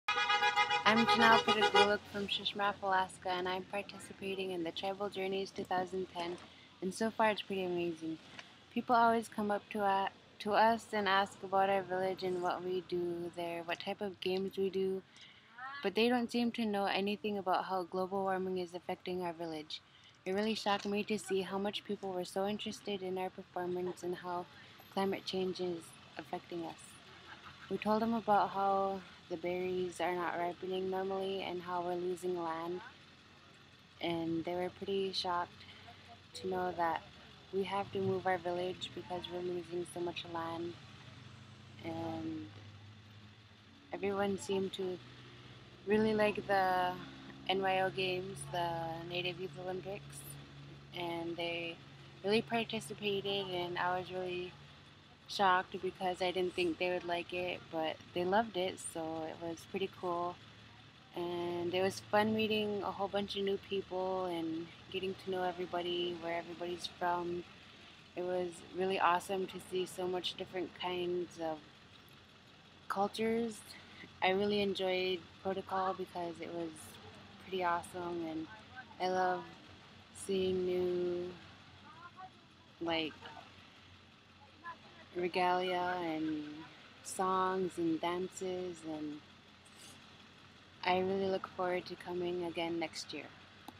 Journey to Makah 2010: Janelle Pootoogooluk's experience
The Journeys to Makah 2010 was an event designed to emphasis the importance of Indigenous cultural knowledge, acknowledging inter-tribal connections, honouring elders, and the combination of physical, spiritual, and mental well-being. The Makah Tribal Council, along with the Makah Canoe Society, hosted the 2010 Tribal Journeys and invited all participants to Makah Territory in Neah Bay, WA - the furthest northwest tip on the lower 48 United States.

In this video, student Janelle Pootoogooluk talks about her experience on the trip.